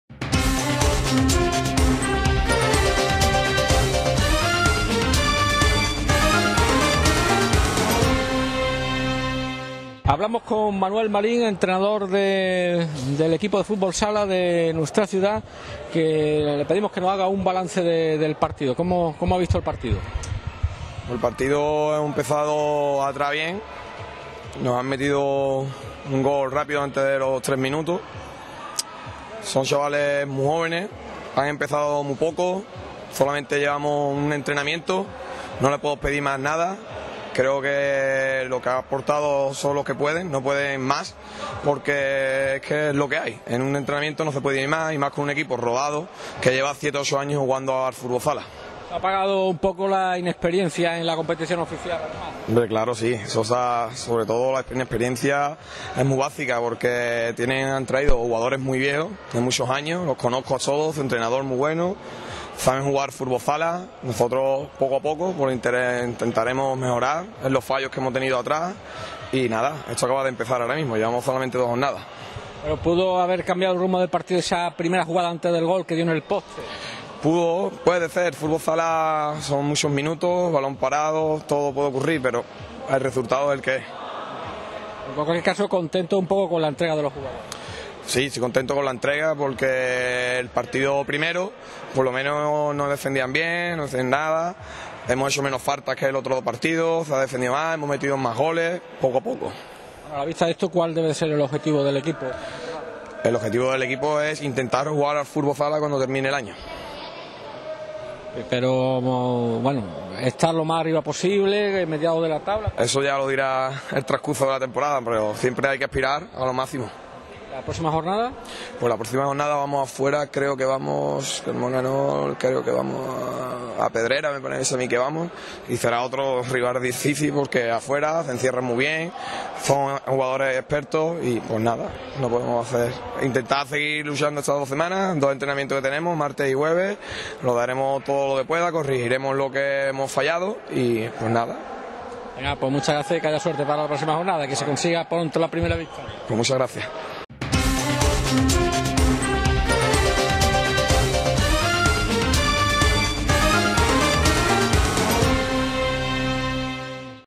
UTRERA - ENTREVISTA STA AL ENTRENADOR DEL EQUIPO DE FÚTBOL SALA C. D. LAS TORRES UTRERA 08
Entrevista al entrenador del equipo de fútbol salsa C. D. Las Torres Utrera 08, Jesus Miguel Miguelez. --